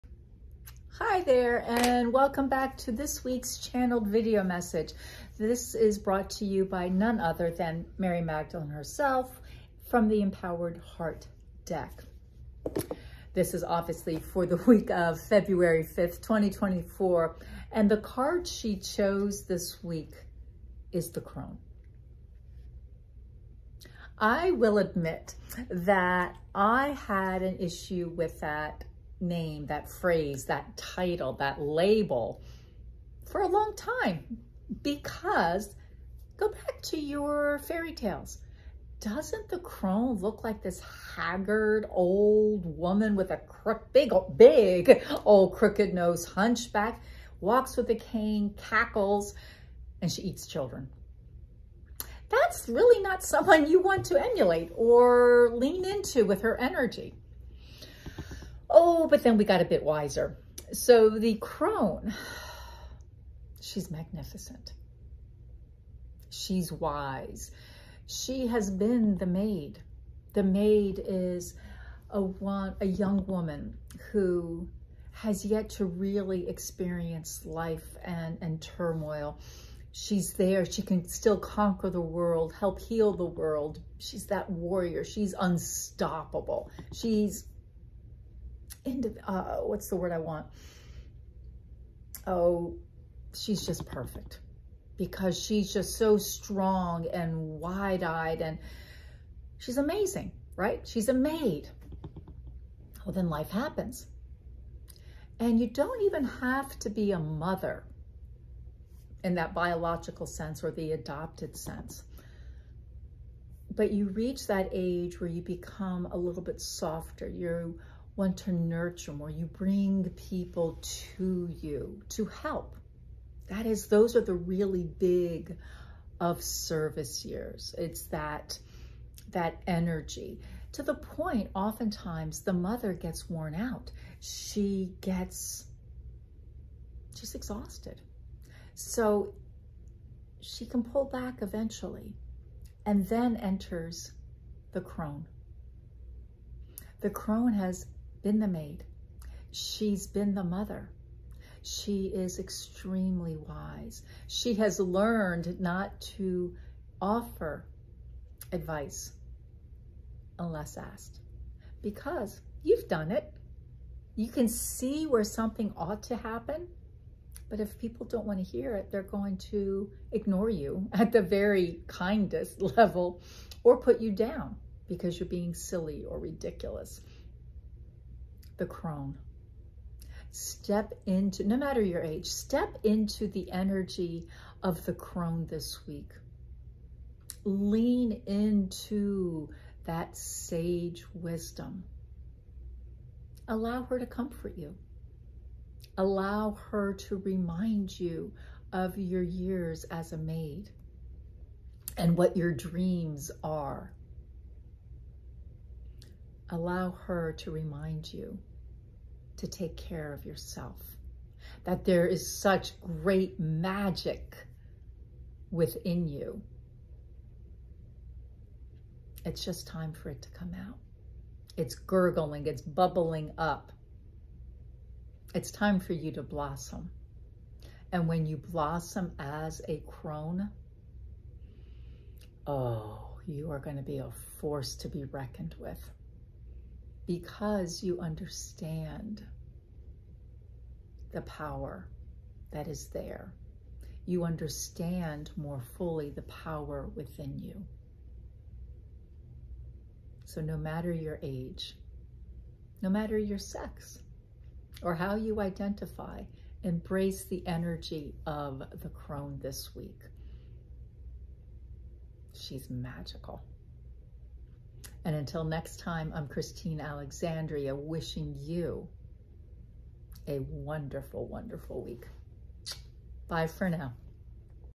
Channeled Message for the week of February 4, 2024
Using a card from the Empowered Heart Deck, lean to hear with the Magdalene wishes to share regarding The Crone.

Lean in for this week's channeled message.